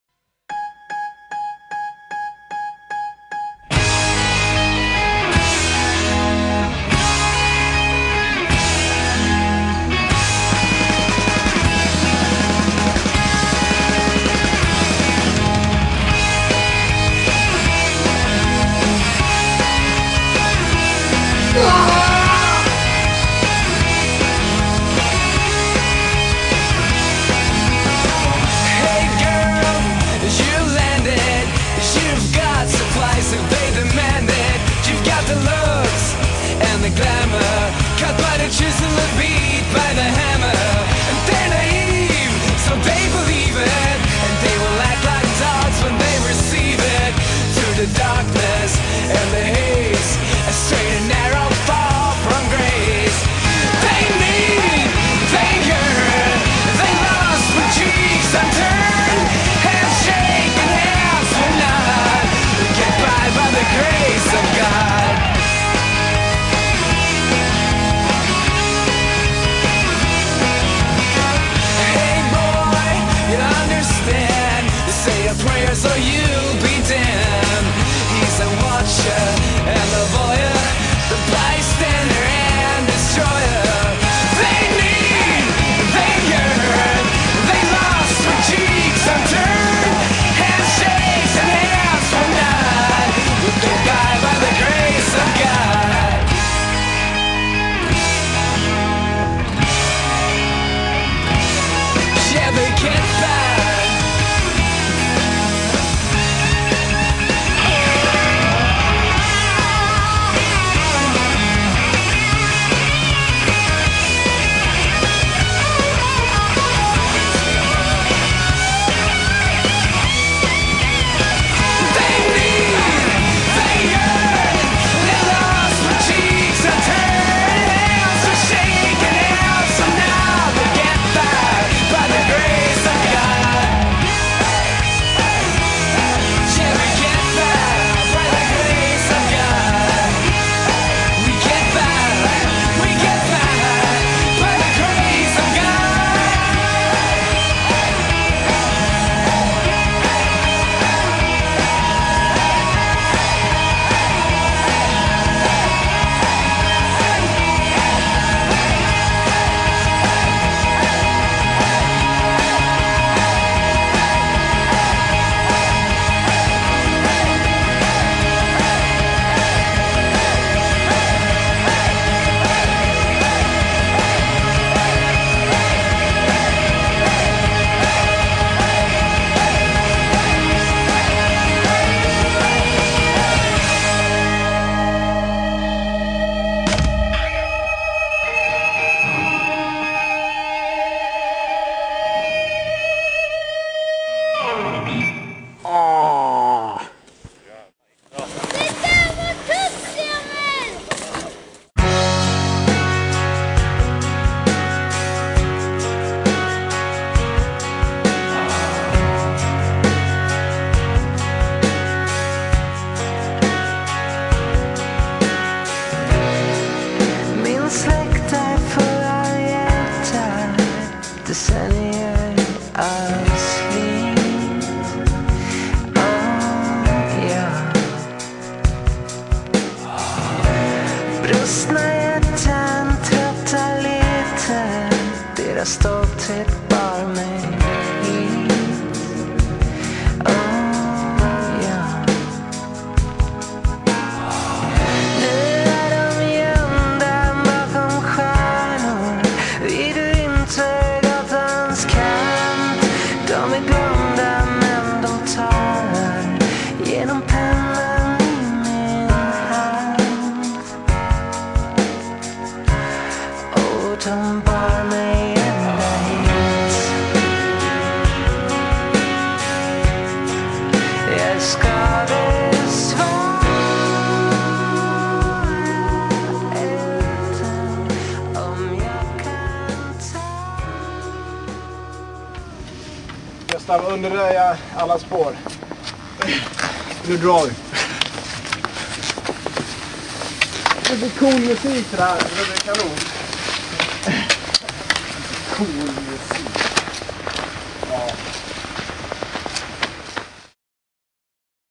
Not Another Cross Hill Project Part 2 (cross country ski movie)
A Cross country skiing movie produced in Mora in 2006.